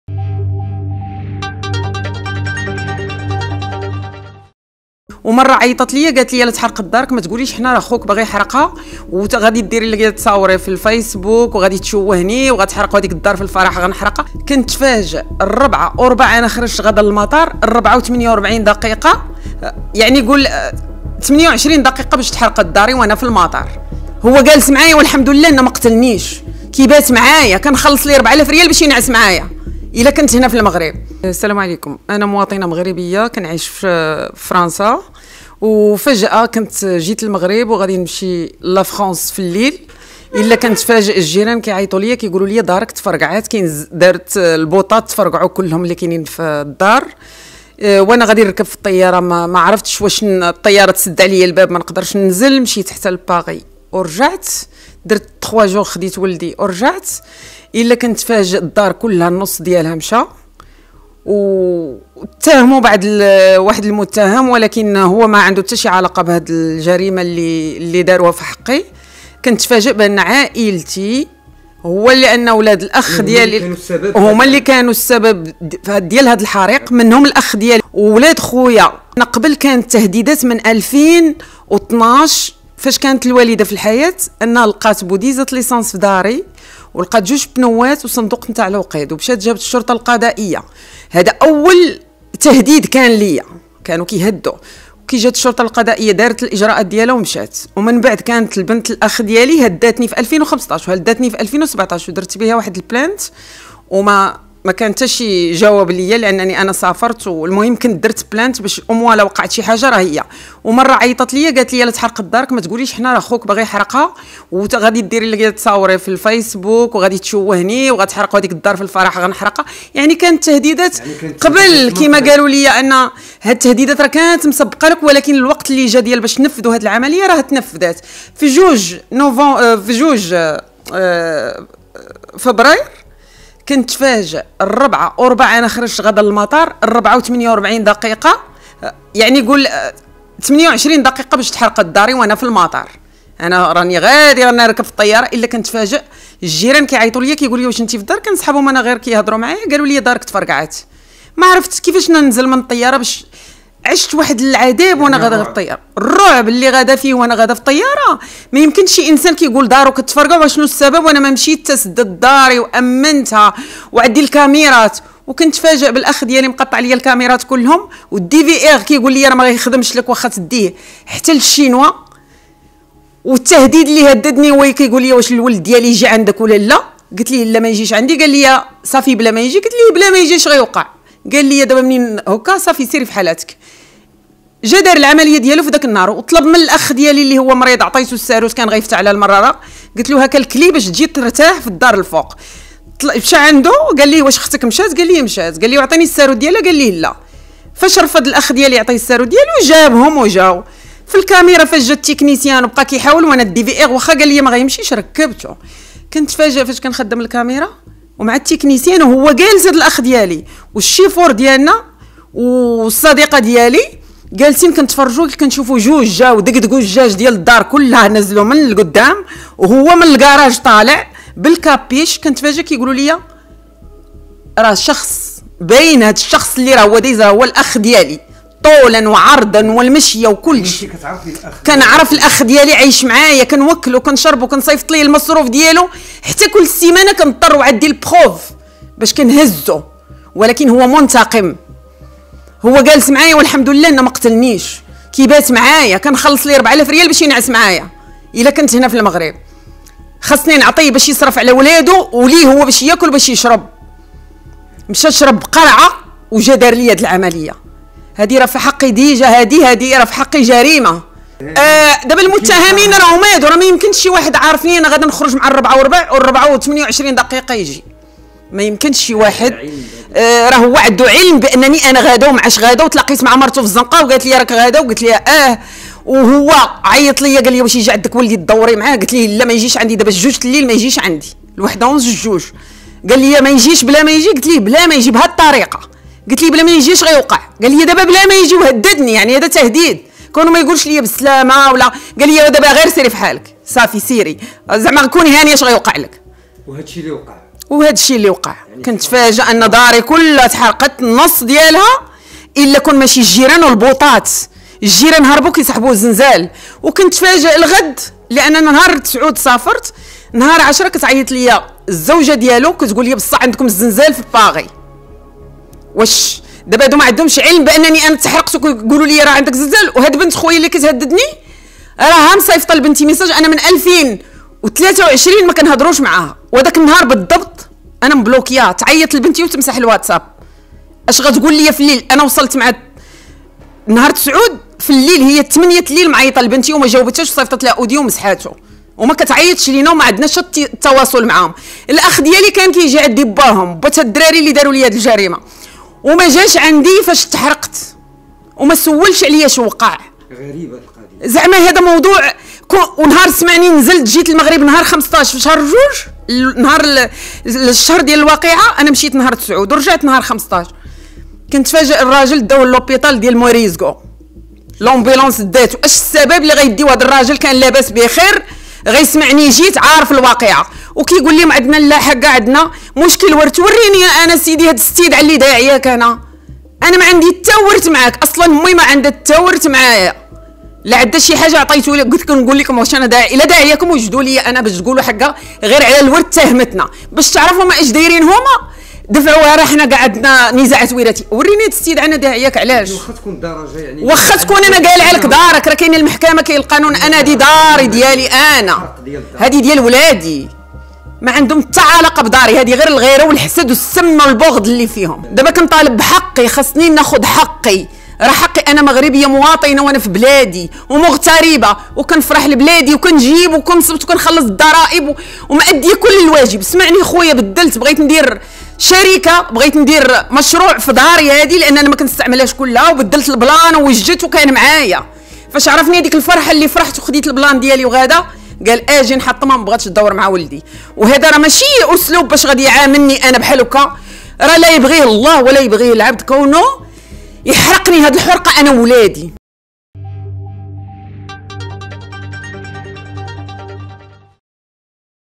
مابقات خاوة ...مواطنة من مغاربة العالم كتهم خوها بحرق الفيلا ديالها "
اتهمت أخت أخوها بارتكابه جريمة في حقها وهي حرق منزلها بالكامل